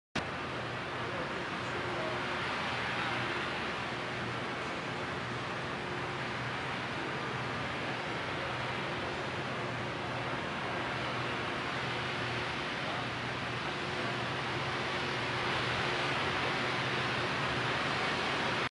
Super Rigs
Hurricane damaged oil rigs stand in the ocean.